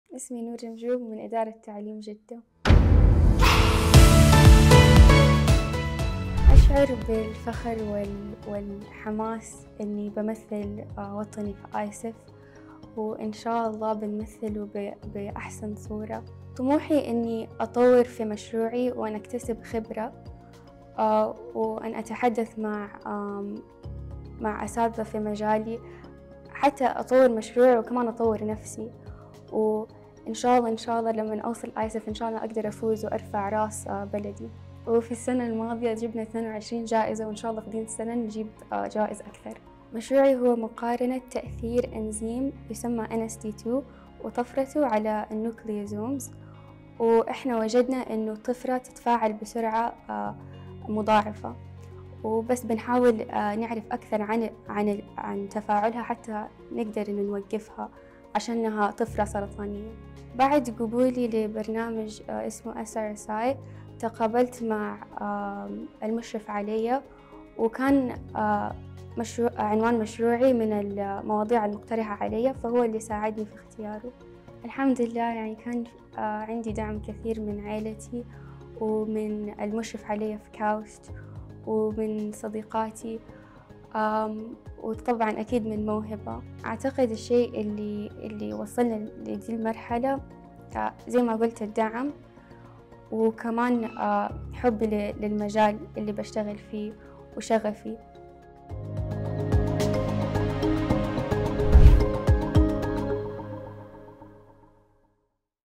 الطالبة نور جمجوم
"حبي للمجال الذي أعمل فيه وشغفي أوصلني لهذه المرحلة، وإن شاء الله نحقق هذا العام في آيسف_2023 جوائز أكثر مما تحقق العام الماضي"

الطالبة نور جمجوم من إدارة تعليم جدة